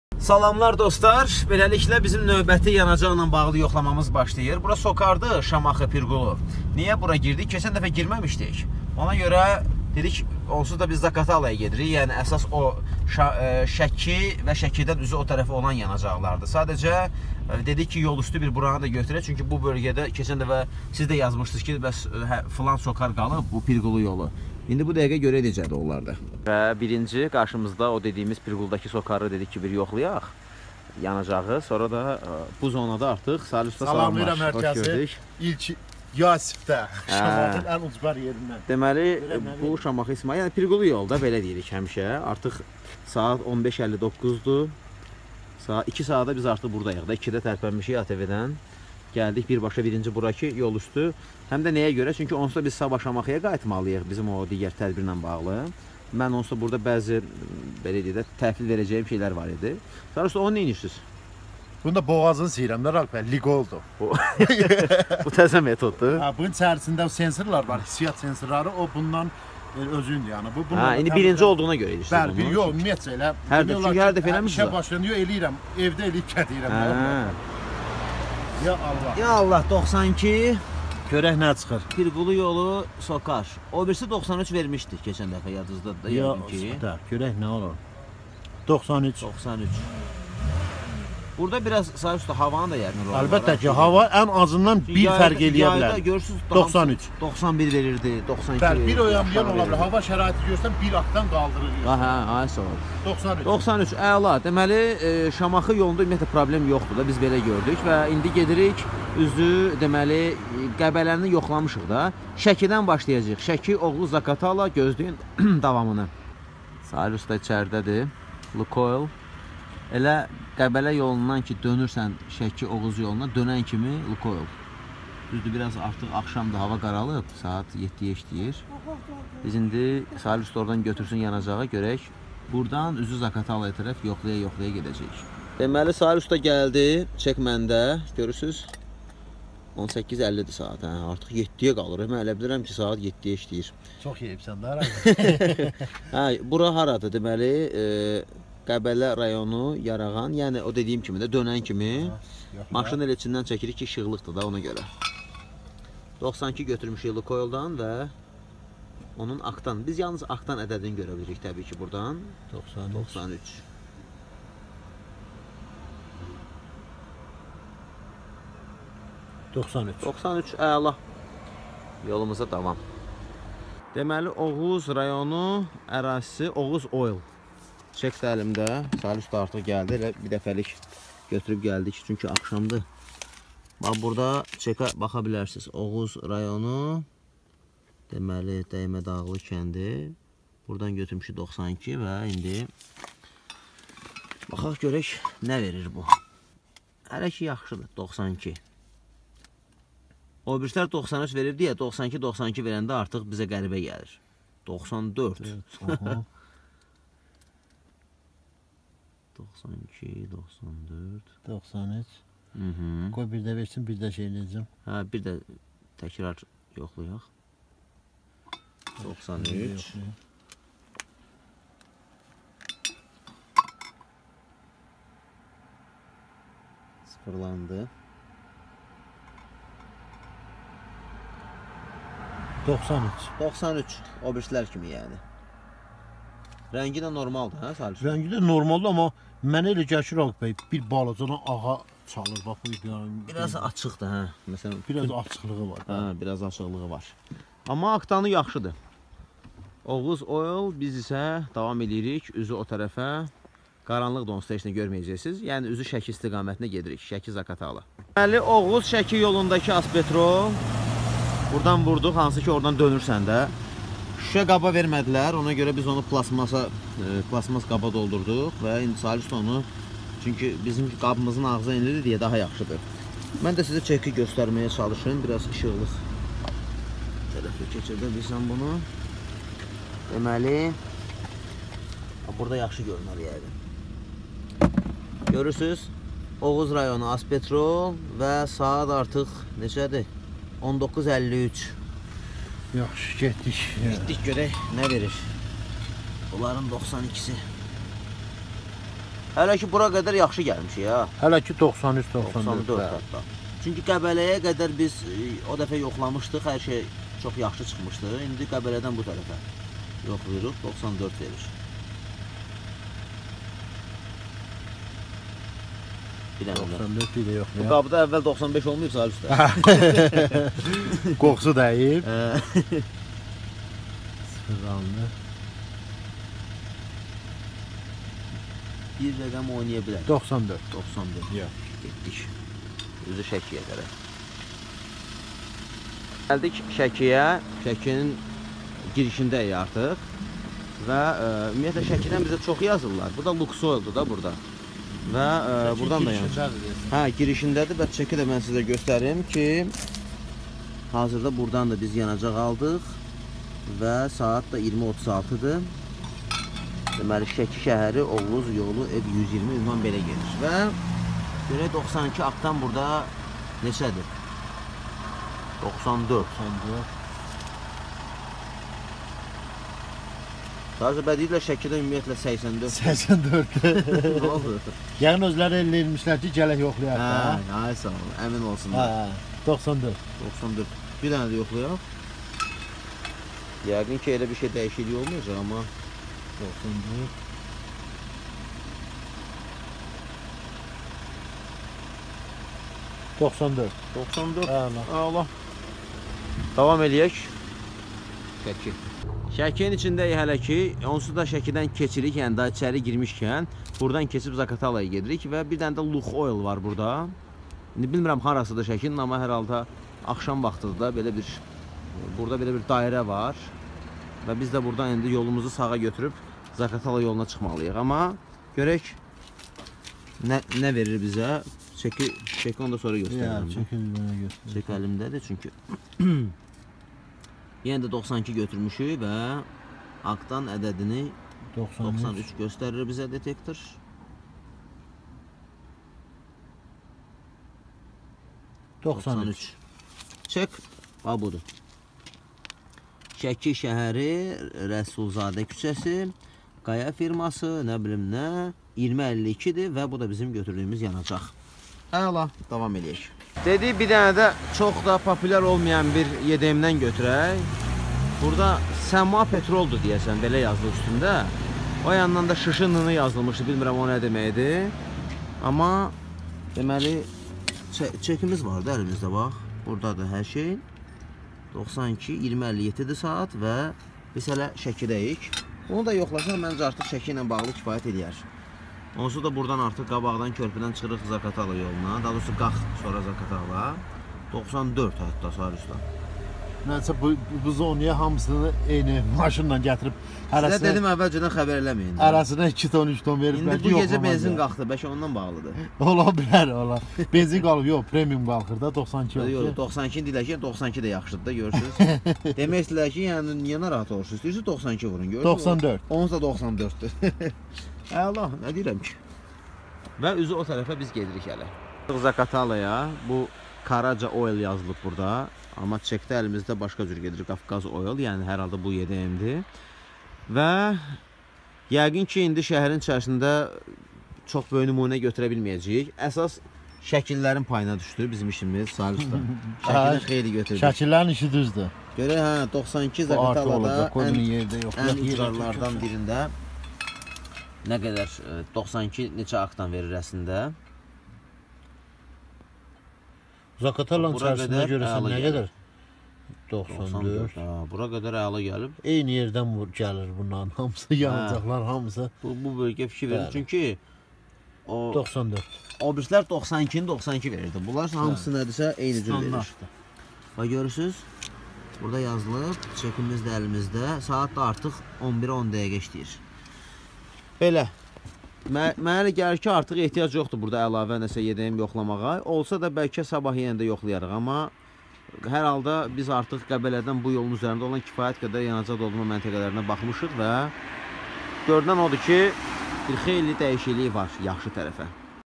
Qebele-Zaqatala yolu yanacaqların oktan veziyyeti (Ferdi yoxlama)
Şamaxı Socar, ve Oğuz, Şeki, Zaqatala yolunda olan esas YDMler.